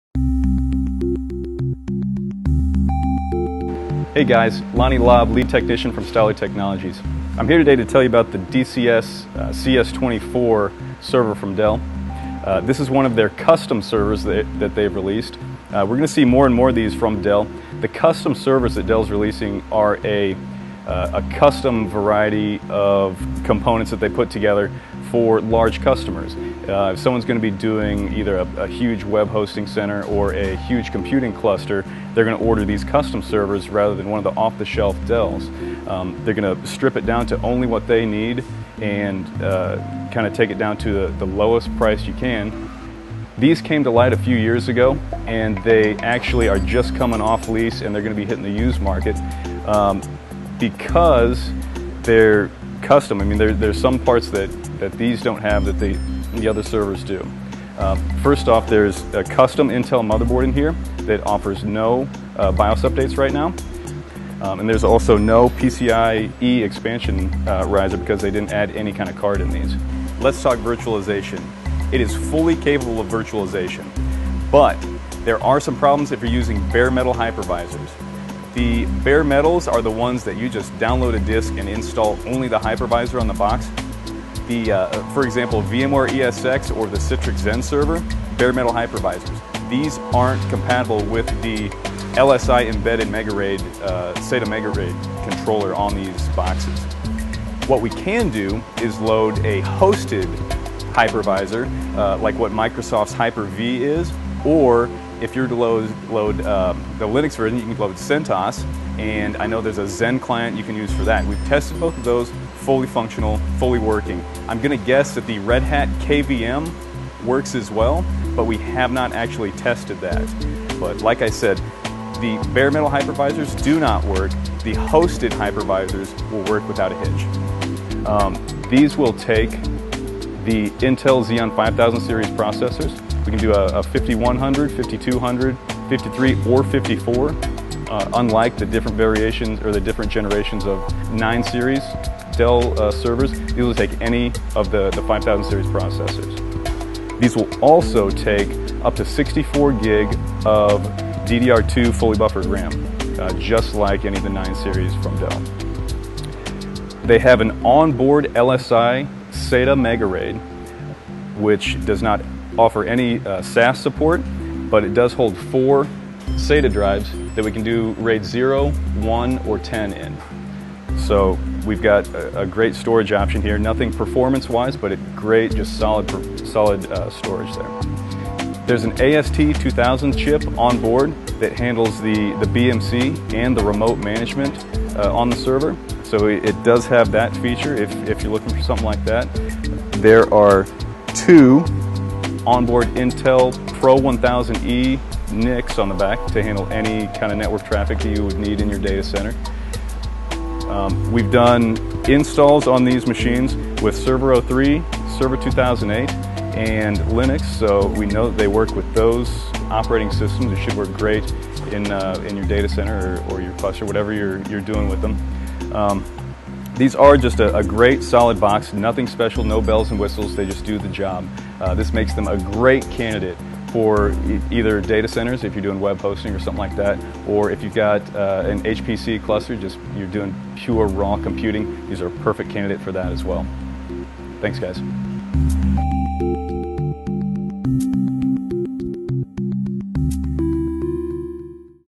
Dell DCS24 Custom Server - System Overview
*UPDATED WITH VIRTUALIZATION* - These DCS24 units are a powerful and inexpensive solution for large data center use. With up to 8 CPU Cores, 64GB of RAM, and 8TB of storage per unit, these data center servers offer a tremendous flexibility. They make a perfect candidate for Web Hosting Data Centers or High Performance Computing Clusters (HPCC).

Helpful features include:

- Low-cost, highly rack dense
- All the essentials for large-scale rollouts
- Intel motherboard specifically designed for this server